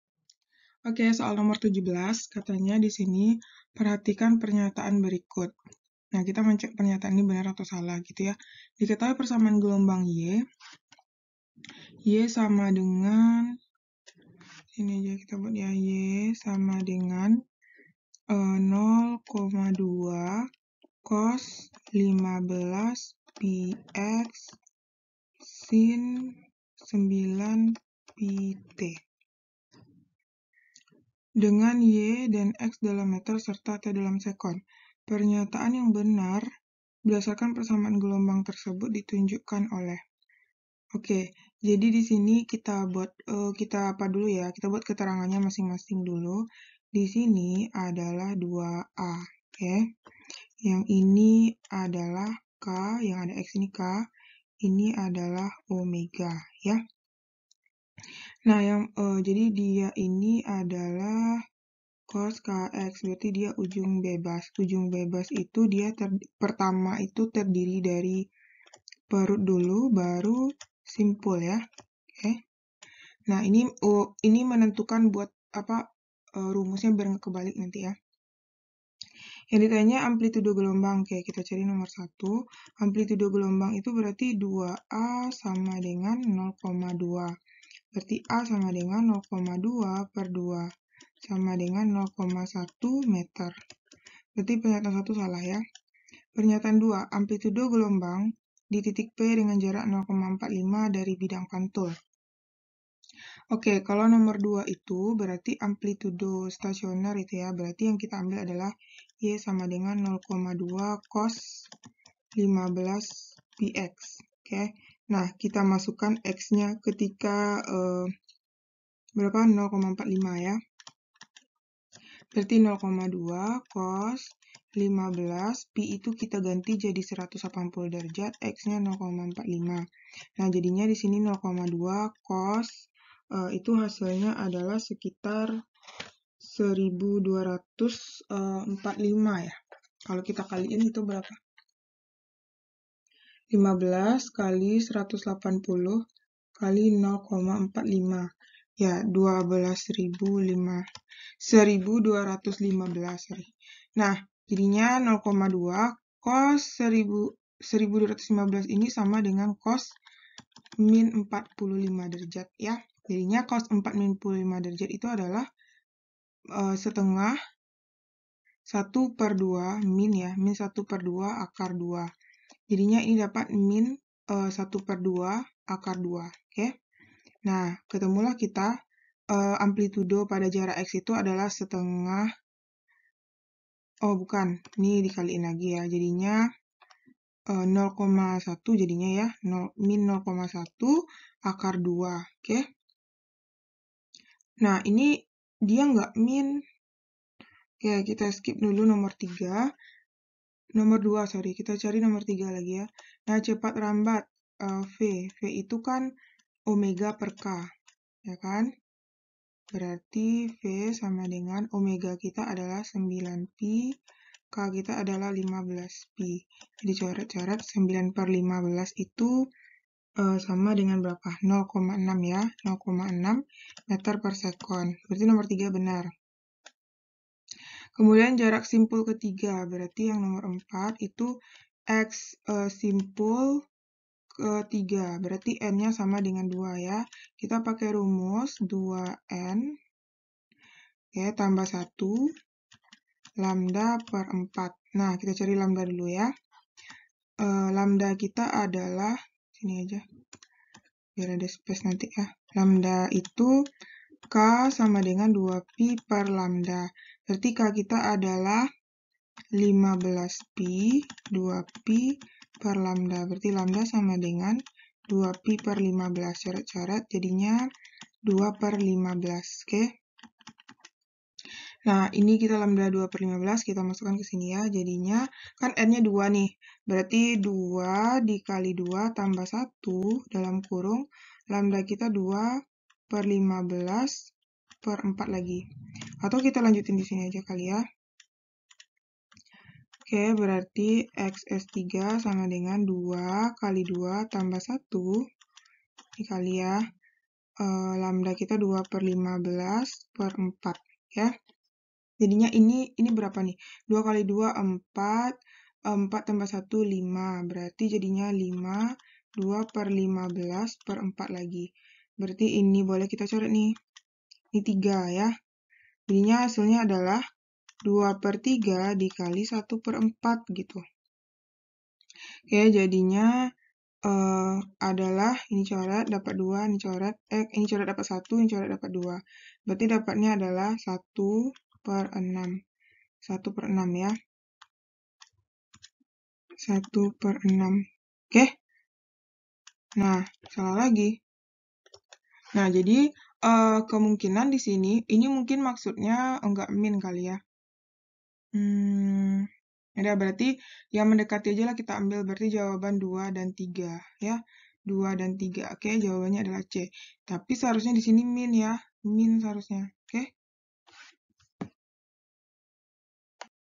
PH GELOMBANG BERJALAN & STASIONER no 17 intanpariwara2022
perhatikan pernyataan-pernyataan berikut!
1) amplitudo gelombang adalah 0,2 m
2) amlitudo gelombang di titik P dengan jarak 0,45 m dari bidang pantul adalah 0,1 akar 2 m
3) cepat rambat gelombang adalah 0,6 m/s
4) jarak simpul ke 3 adalah 2 /3 m
diketahui persamaan gelombang stasioner y = 0,2 cos 15 pi x sin 9 pi t dengan y dalam meter serta t dalam sekon. pernyataan yang benar berdasarkan persamaan gelombang tersebut ditunjukkan oleh angka.